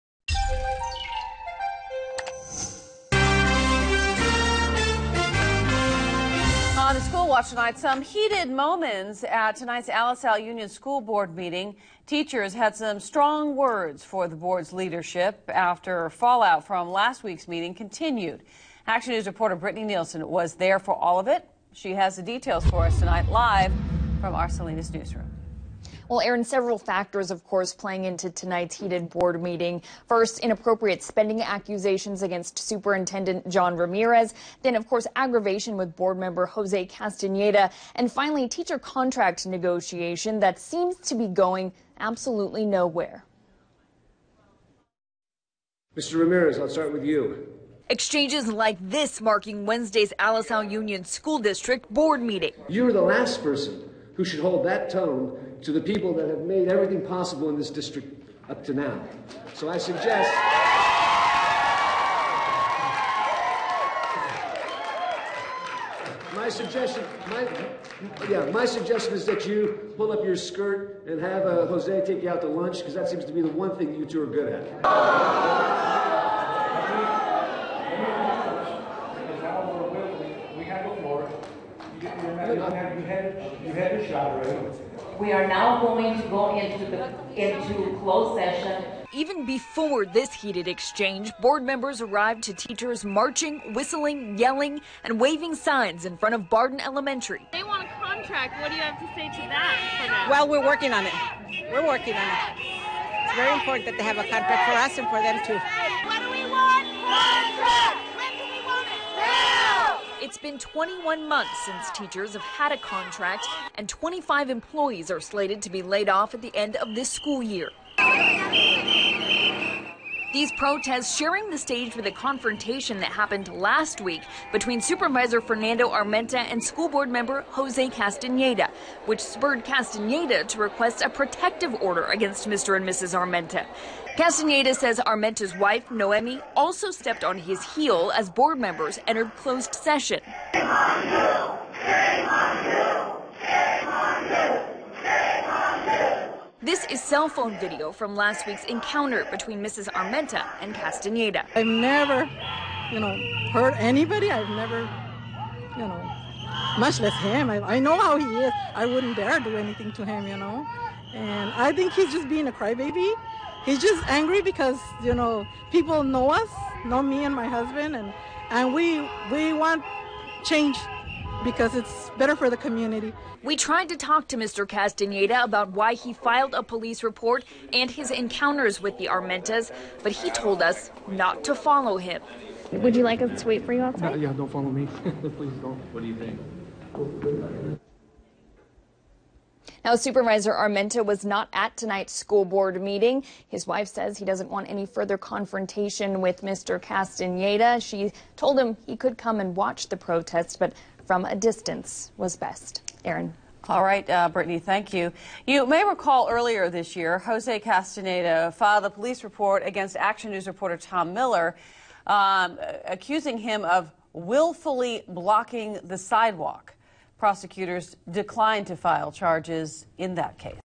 Heated Alisal School Board Meeting
Some heated moments at tonight's Alisal Union school board meeting. Teachers had strong words for the board's leadership after fallout from last week's meeting continued.